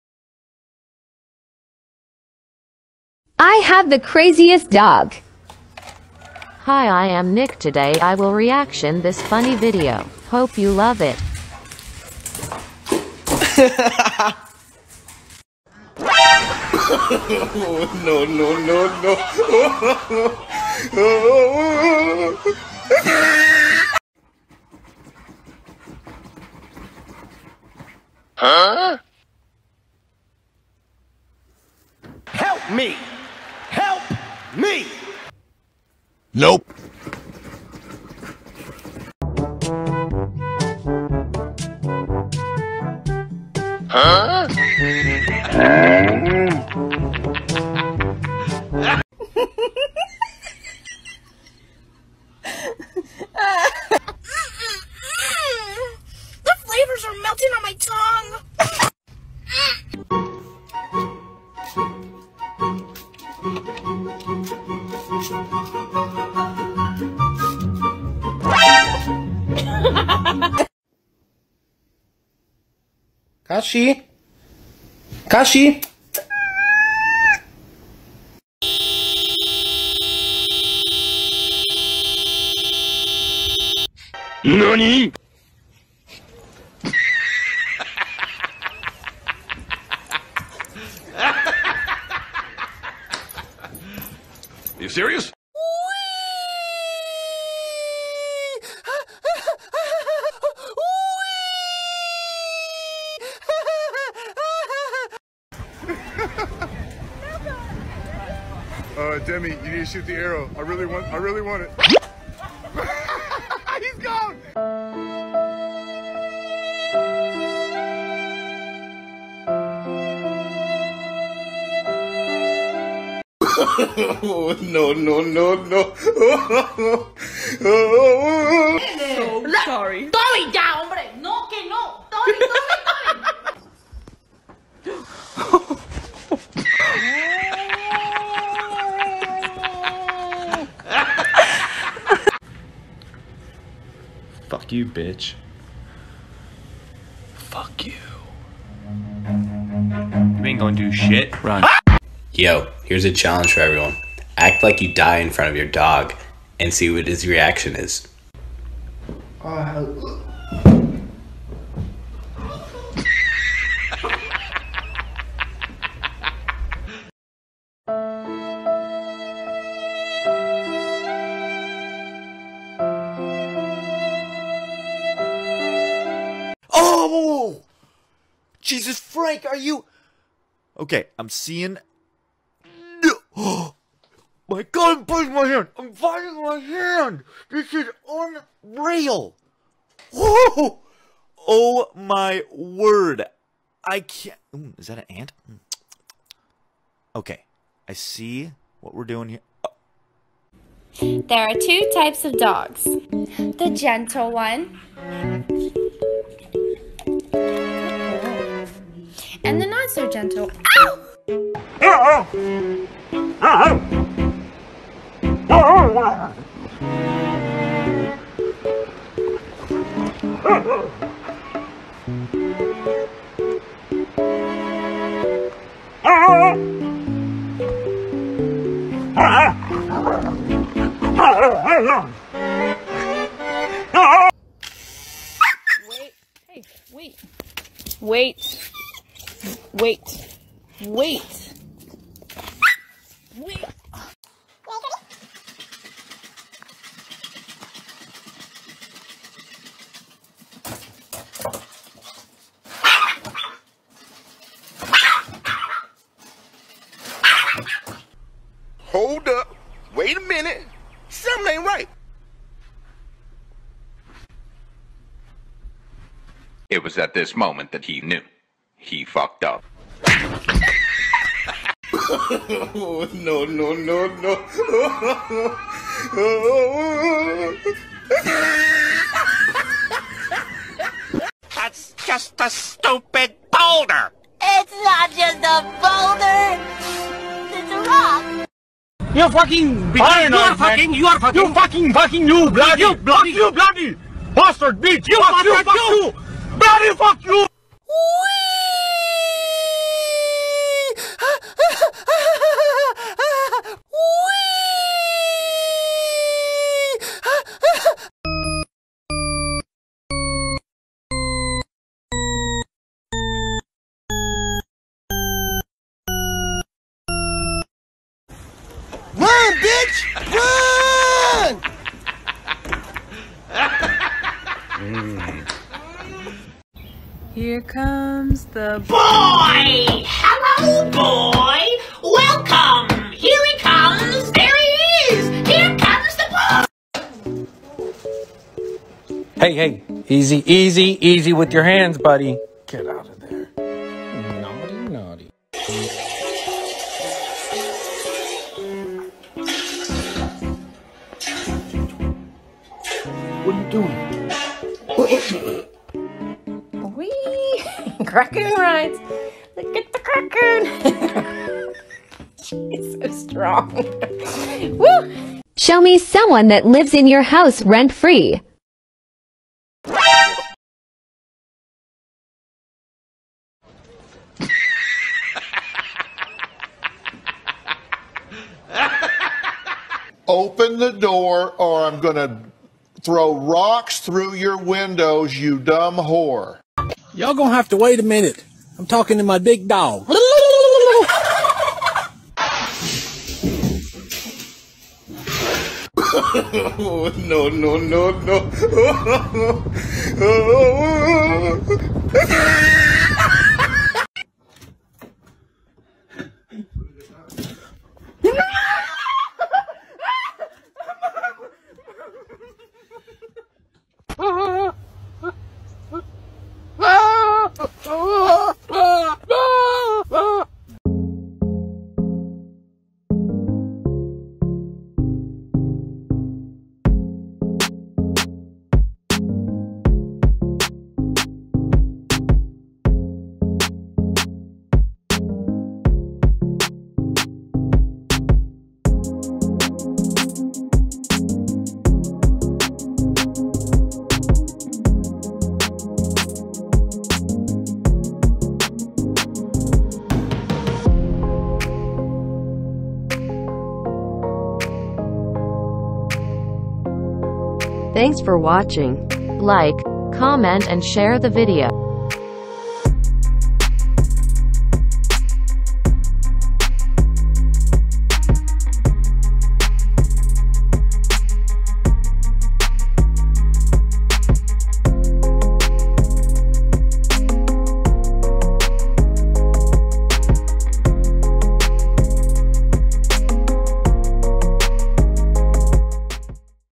Funniest Pet 😹- Best Of The 2022 Funny Pet Videos 😂 Part 2 - Pet Planet
Enjoy a new funniest and very cute compilation of the week about a try not to laugh funny animals' life video.
All their breeds differ by size, features, color, temperament.
🌈 Welcome to the Pet Planet channel!

Our purpose, when making compilations is to share videos that we like with other people.